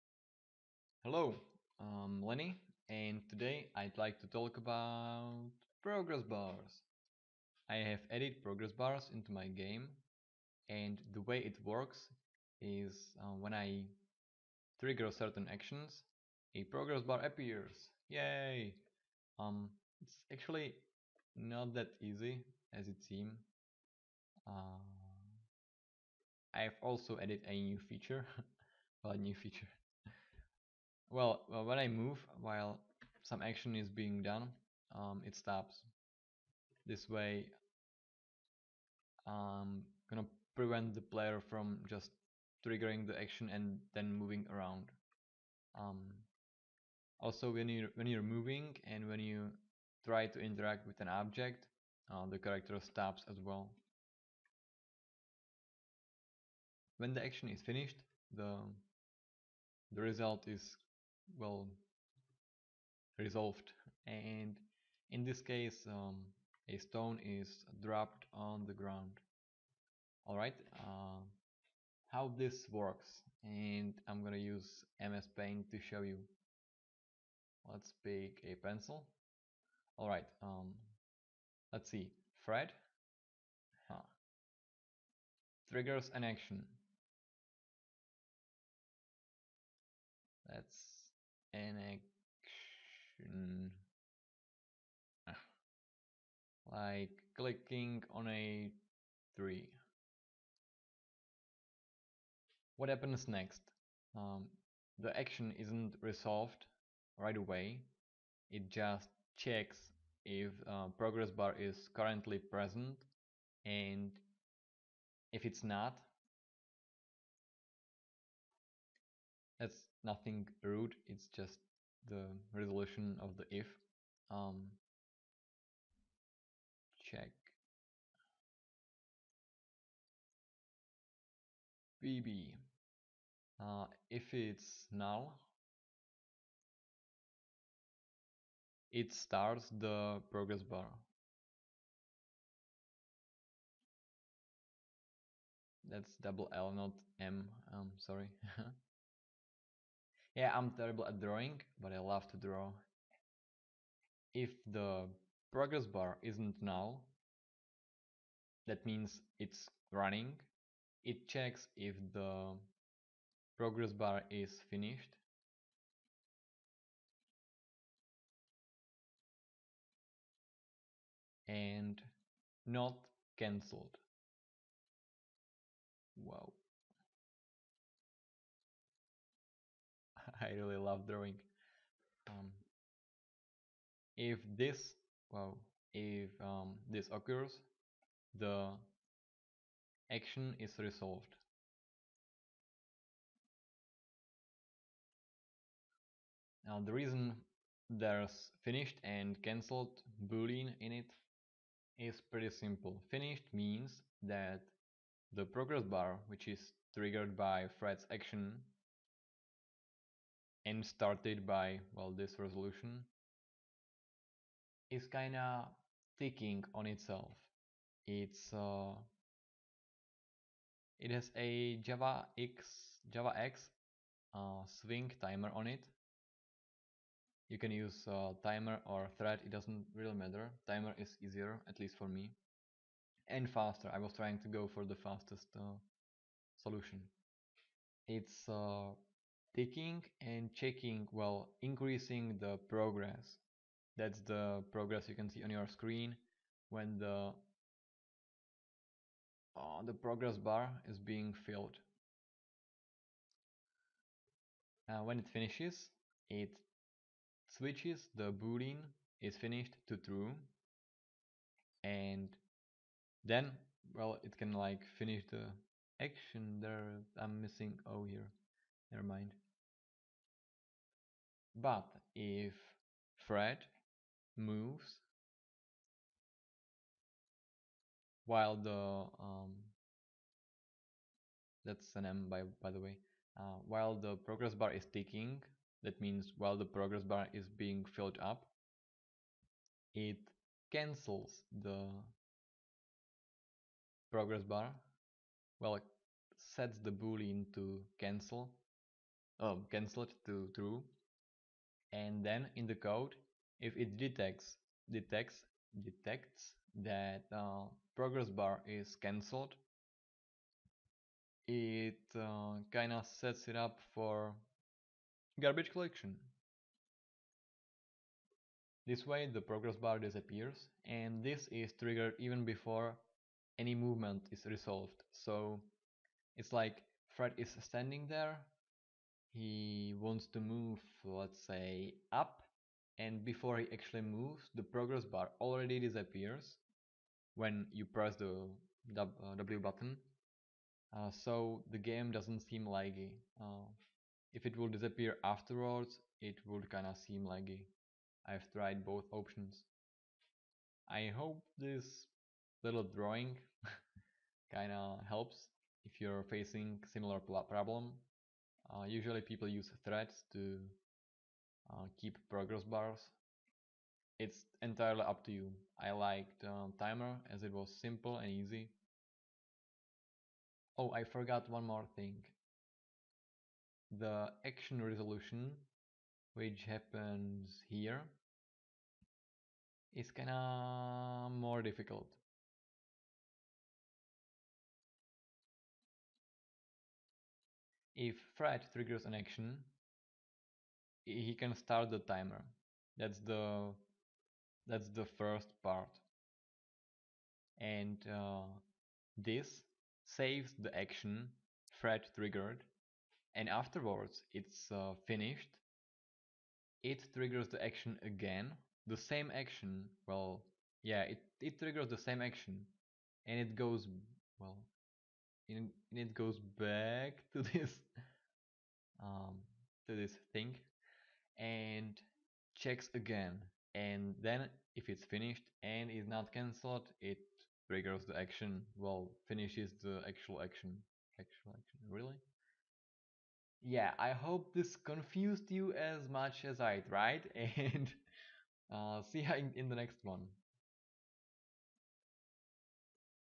Java Netbeans Game Dev Log - part 13: Progress bars!
In this one I've added progress bars and tried to explain them with the help of MS Paint! :))

2,8 k lines of code